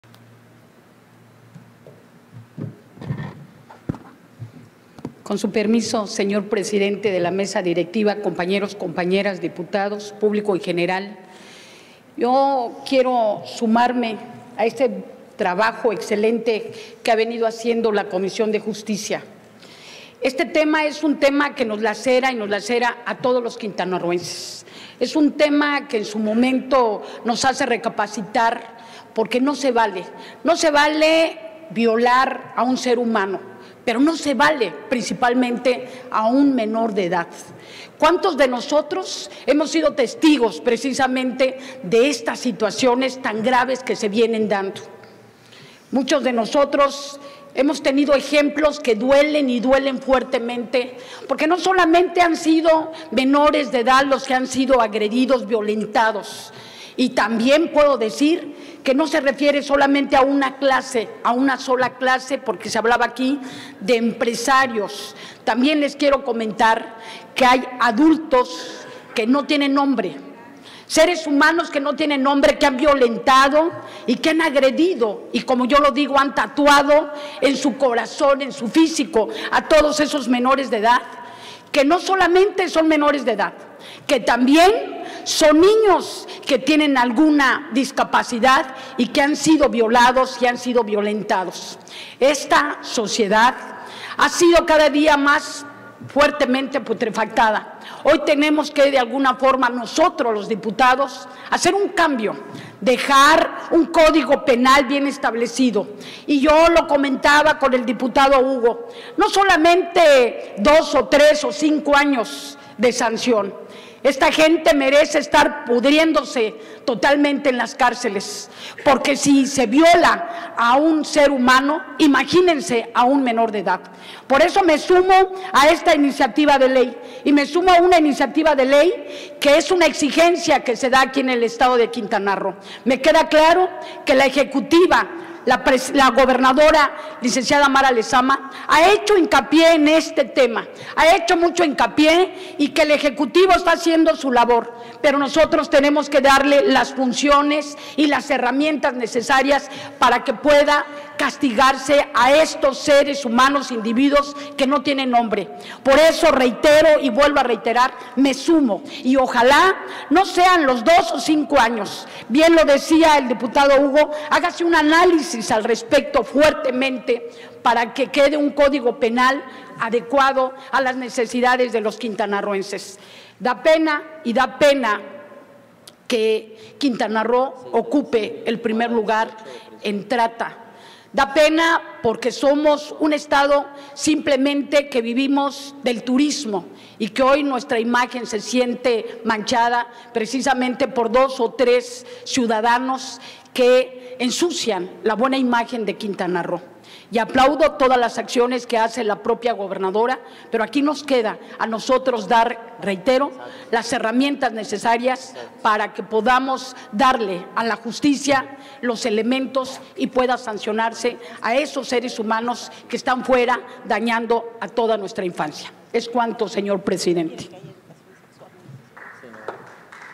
Intervención Dip. Susana Hurtado Vallejo. PVEM (18/Septiembre/2023)
Intervención de la Dip. Susana Hurtado Vallejo, del PVEM, en relación al  Dictamen con Proyecto de Decreto por el que se reforman los artículos 78 bis, primer párrafo del artículo 129, primer párrafo del artículo 191, y se adicionan el Capítulo II Bis denominado “Pederastia”, los artículos 129 bis y 129 ter, todos del Código Penal para el Estado Libre y Soberano de Quintana Roo, para su aprobación, en su caso. Publicado en la Gaceta Parlamentaria Año II número 01 Extraordinaria del de septiembre de 2023.